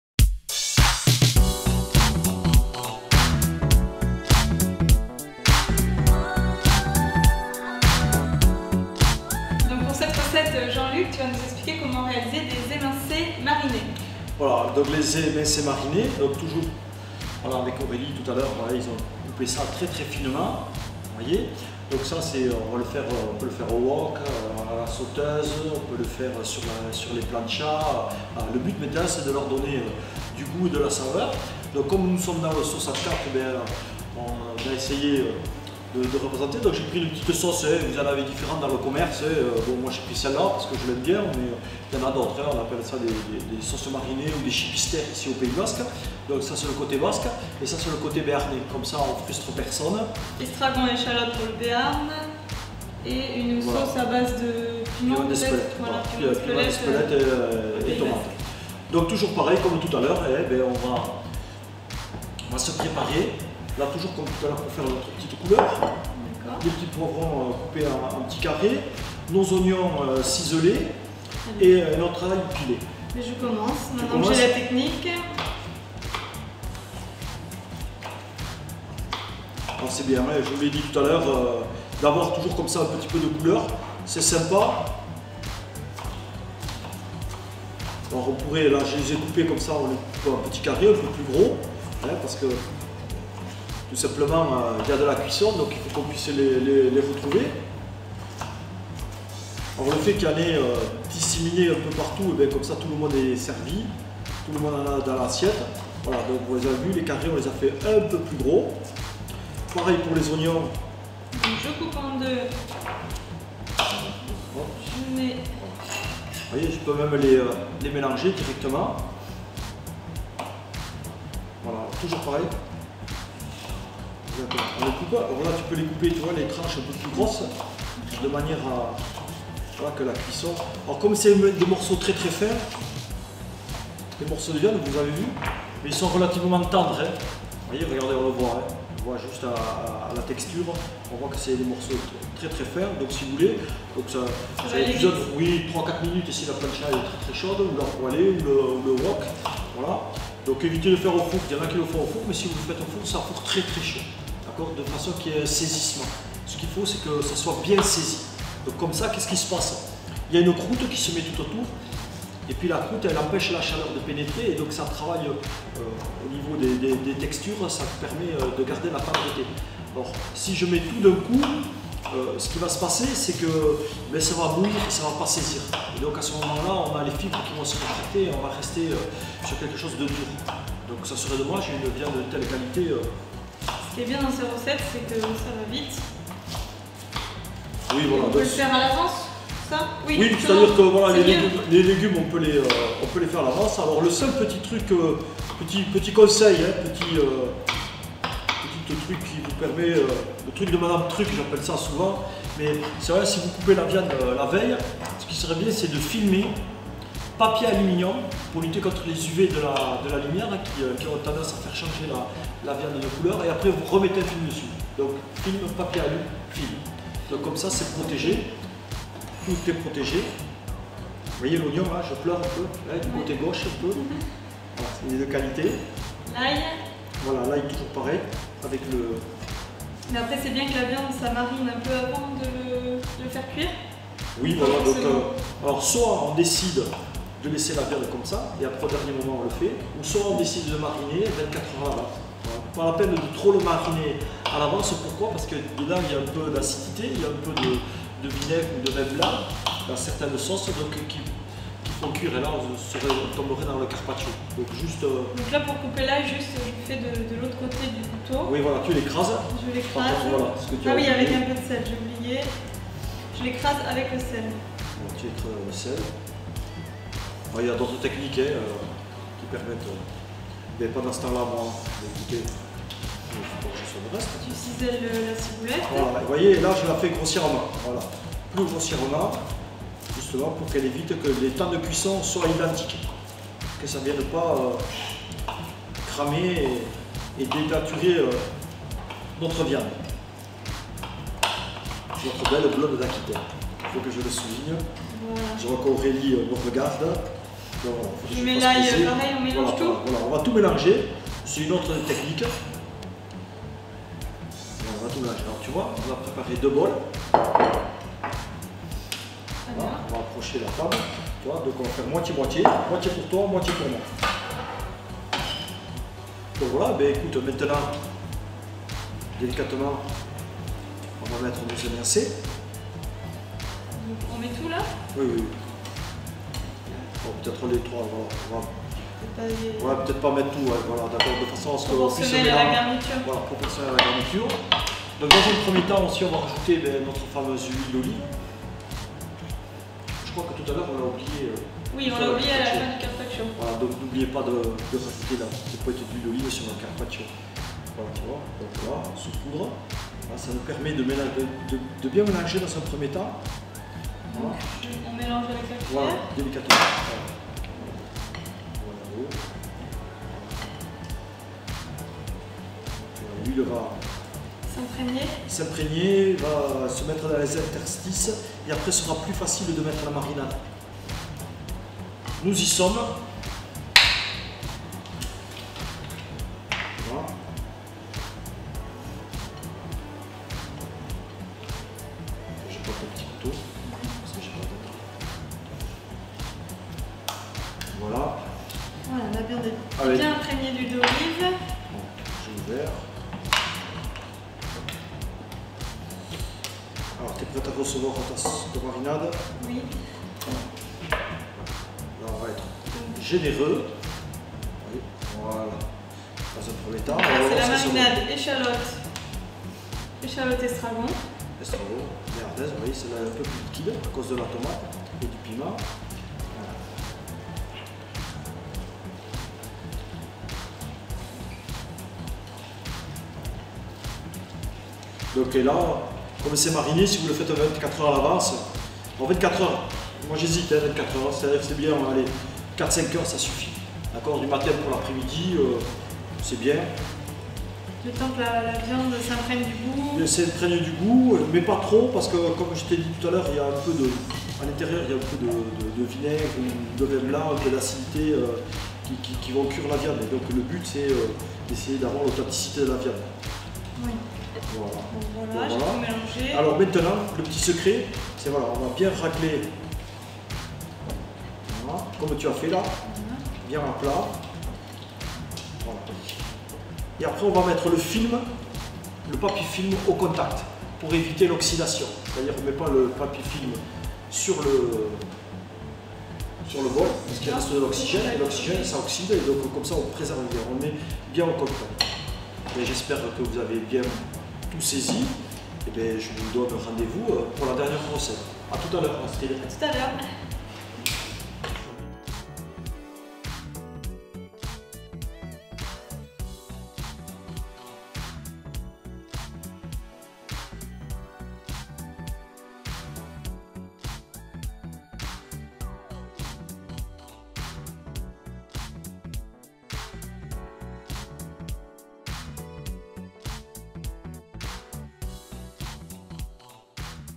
Recette boeuf mariné
Jean-Luc Duvin, formateur charcutier-traiteur à l’Université de Bayonne et Astrid Monteau de la Cité du Goût et des Saveurs poursuivent leurs recettes à base de tranche grasse de Blonde d’Aquitaine. Cette recette vous propose deux marinades, une basquaise et une béarnaise, accompagnées de petits légumes, à faire cuire de différentes façons : plancha, wok, sauteuse. Une recette simple et savoureuse pour l’été !